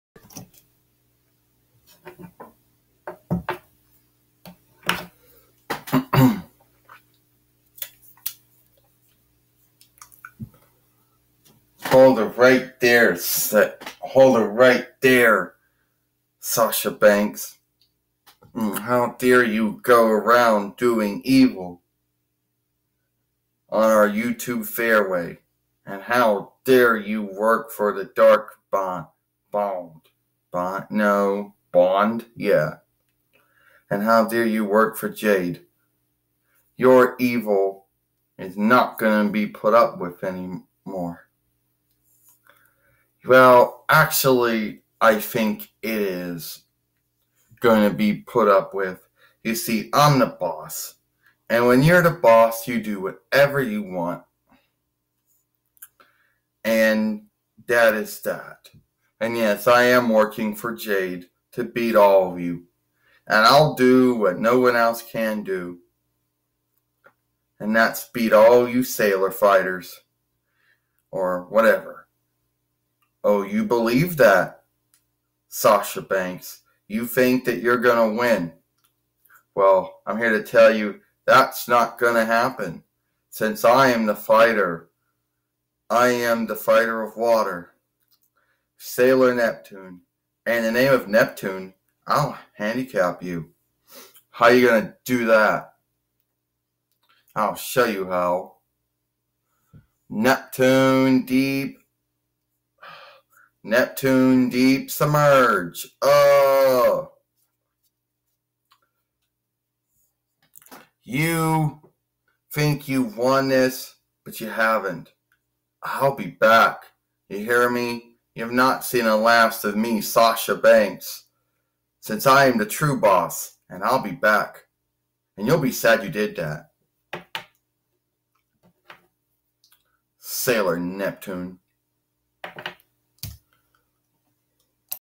SAILOR NEPTUNE FIGHT SASHA BANKS
SAILOR NEPTUNE FIGHT WWE WRESTLER SASHA BANKS WHO IS WORKING FOR DARK BAND AND IS BEAT AND STOP BY SAILOR NEPTUNE